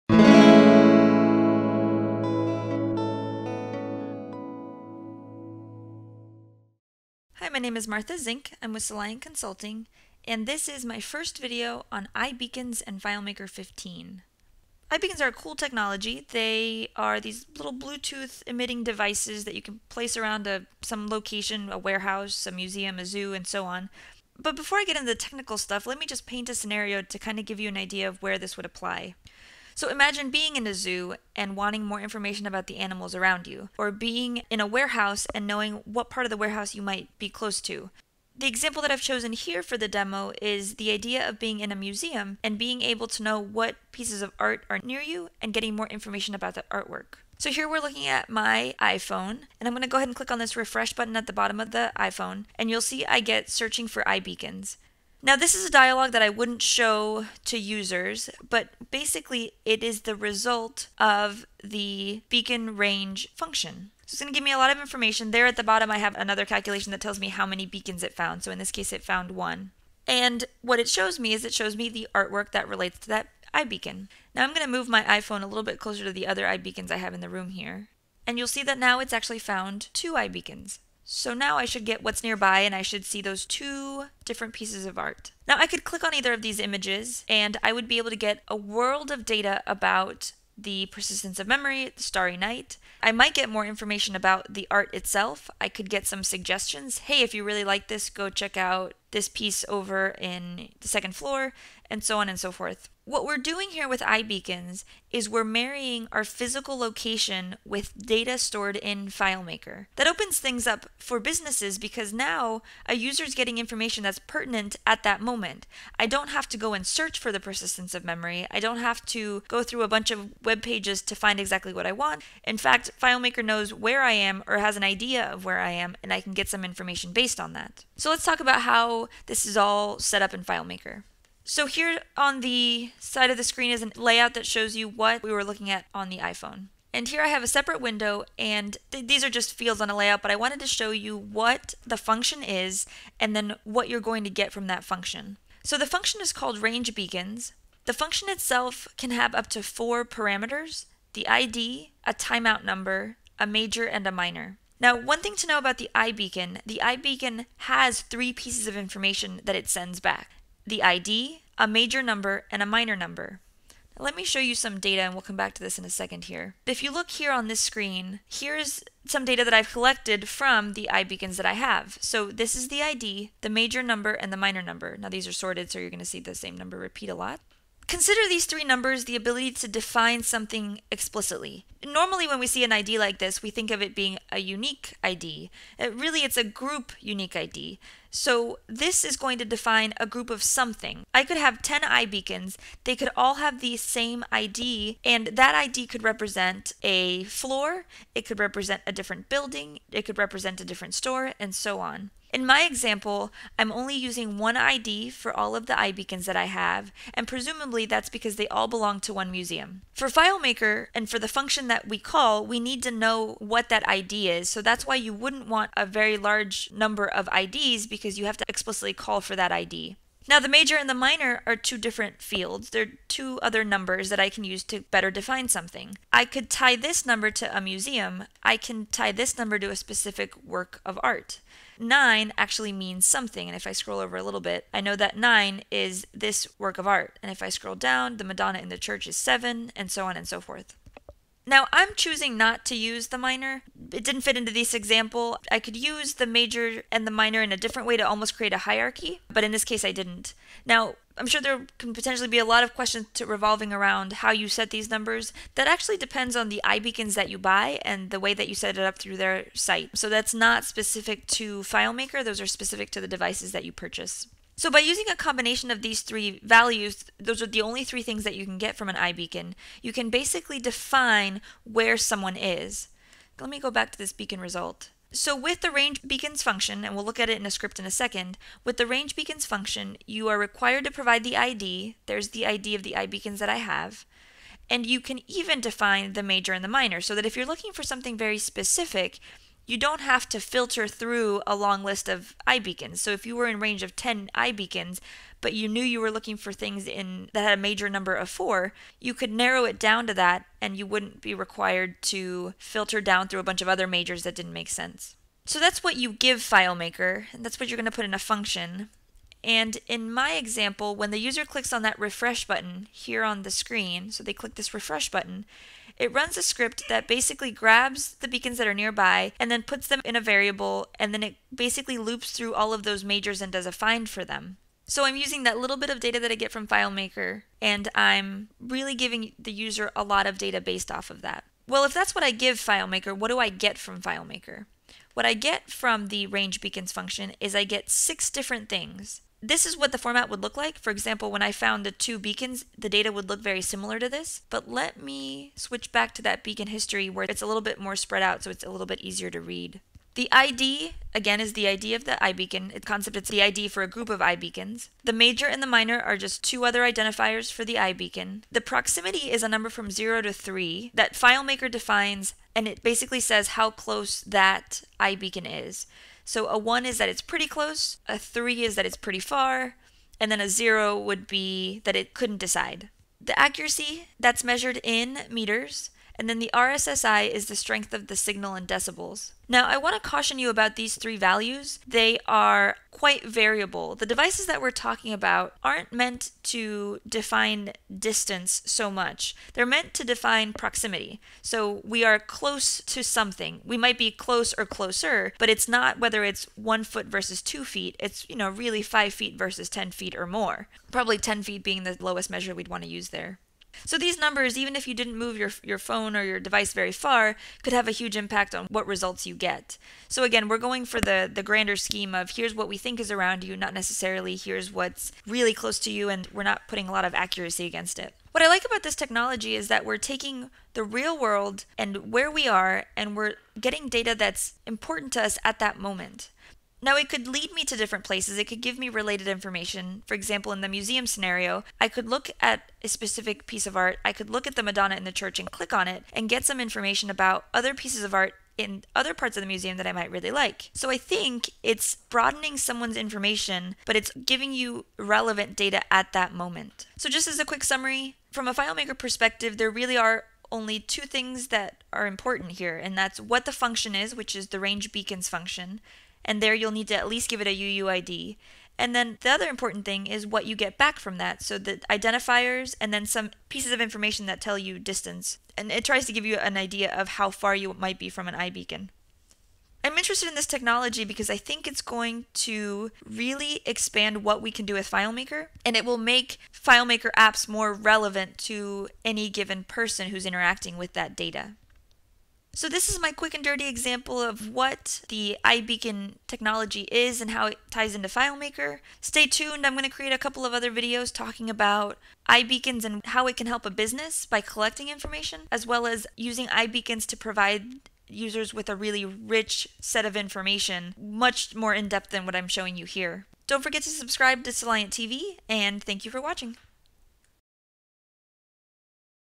FileMaker Tip: Introduction to iBeacons
Martha Zink explains how FileMaker 15 interacts with iBeacons, bringing together physical space with data stored within a FileMaker app. This video shows the new FileMaker function, RangeBeacons, discussing the necessary and optional parameters, as well as the results from the function.